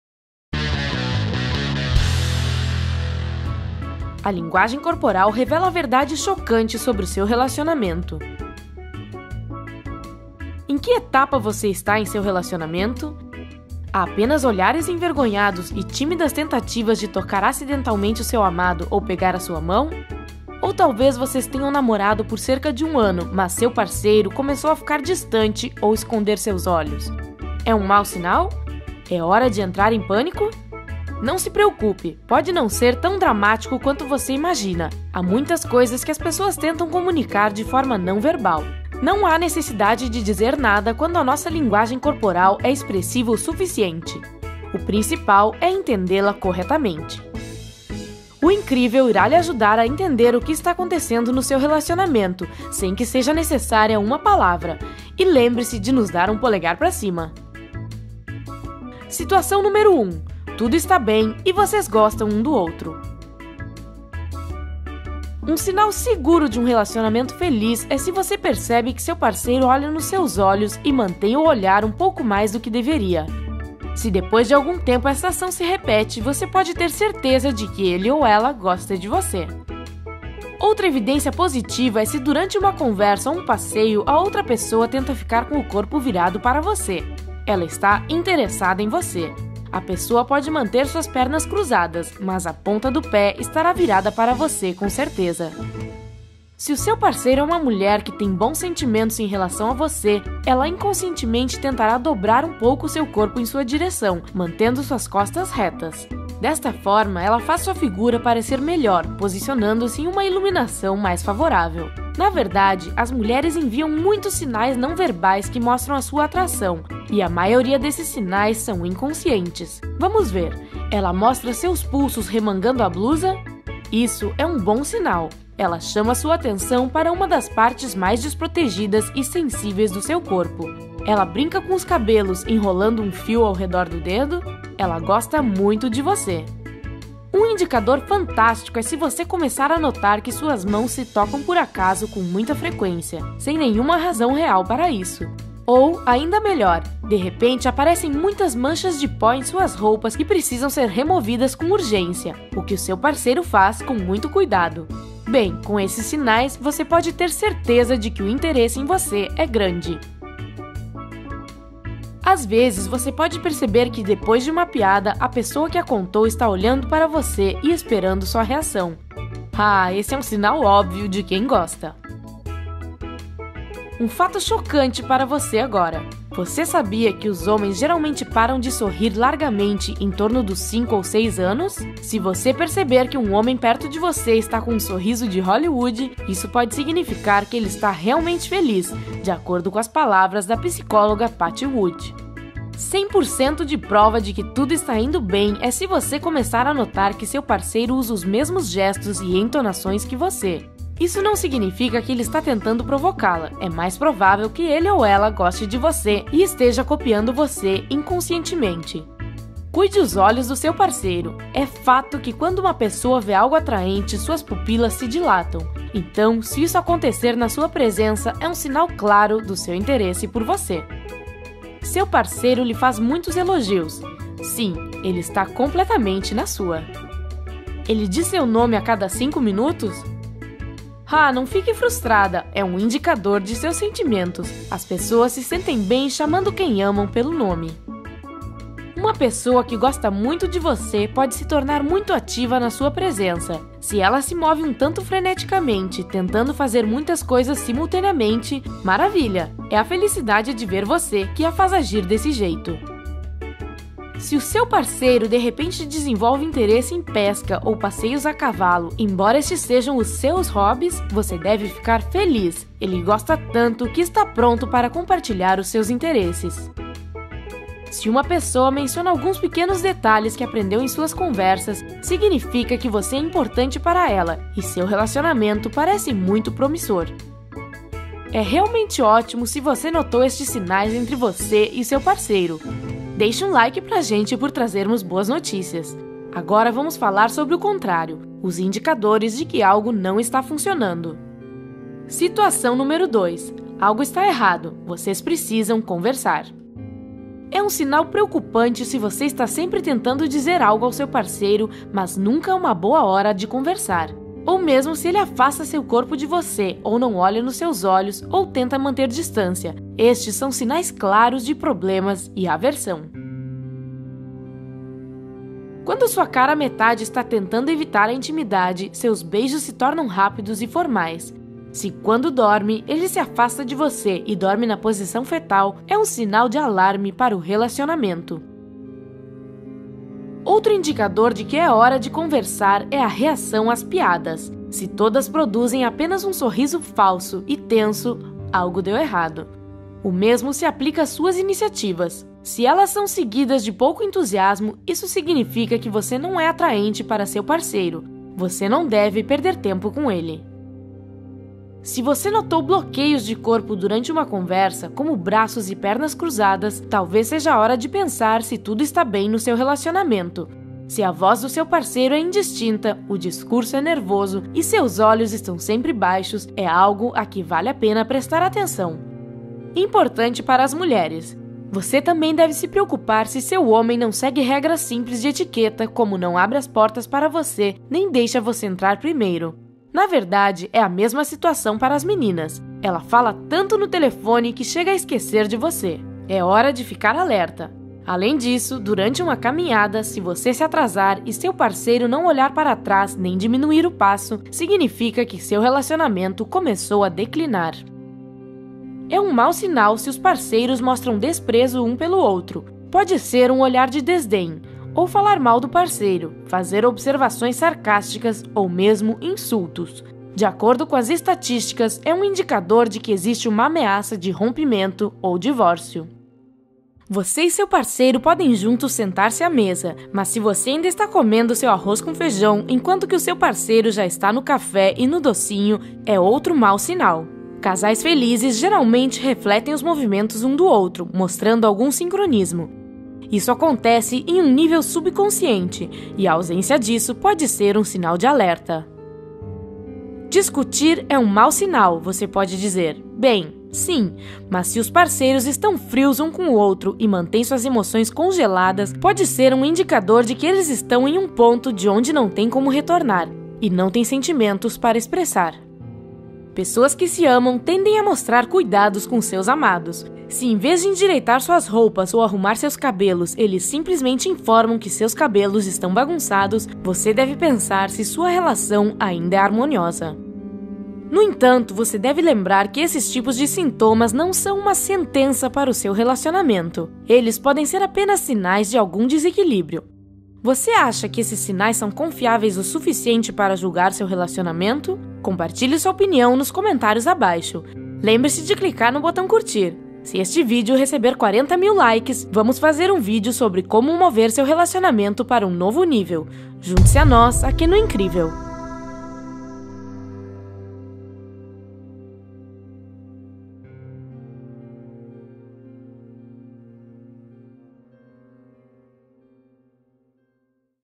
A Linguagem Corporal Revela a Verdade Chocante Sobre o Seu Relacionamento
O que a linguagem corporal diz sobre a sua personalidade e o seu relacionamento. Você entender o que está acontecendo no seu relacionamento sem nenhuma palavra.

5-Minute Crafts Youtube: goo.gl/0f2h9p